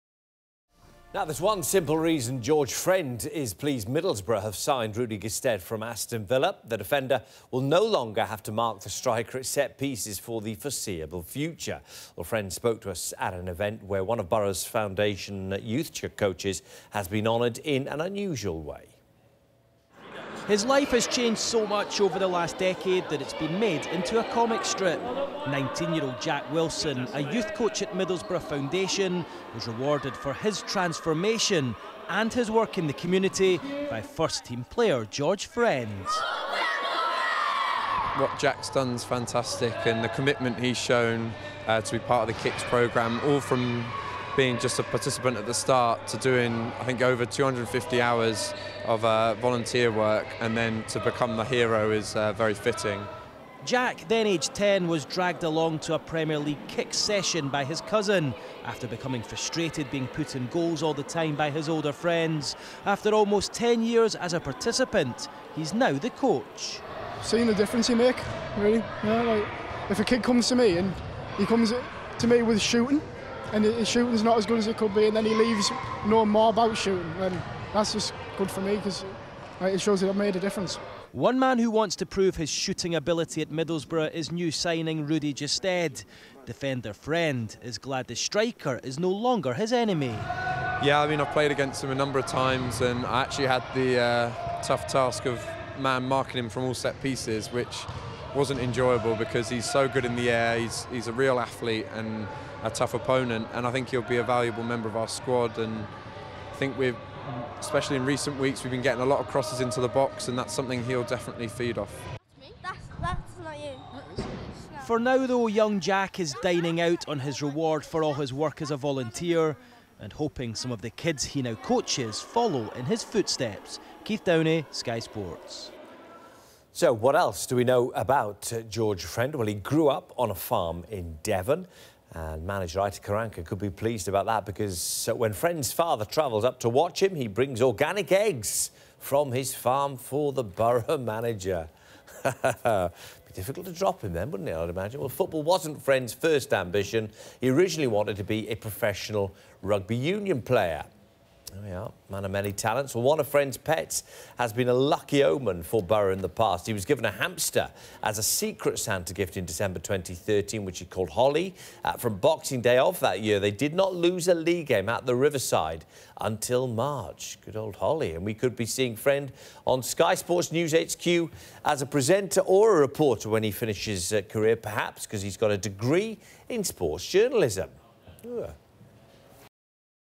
PL Kicks Heroes - Jack Wilson w/George Friend (Sky Sports News Footage)
MFC Foundation PL Kicks coach Jack Wilson is presented with a commemorative comic strip of his development through our Premier League Kicks programme by George Friend. (Footage courtesy of Sky Sports News).